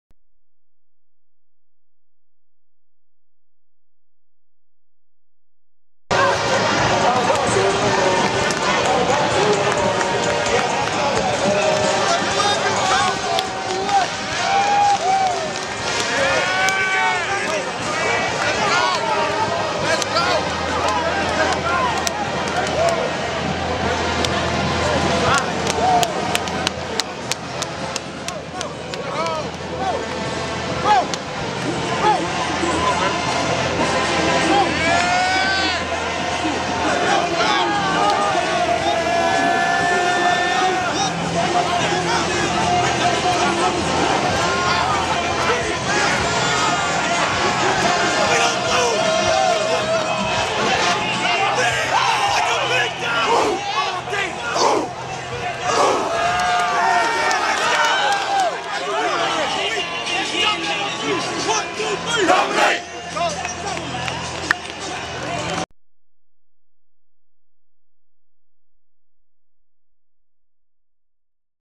LA Tech pre game bark circle vs Texas A&M
LA Tech holds its pre-game Bark Circle prior to the Texas A&M game at Shreveport, LA.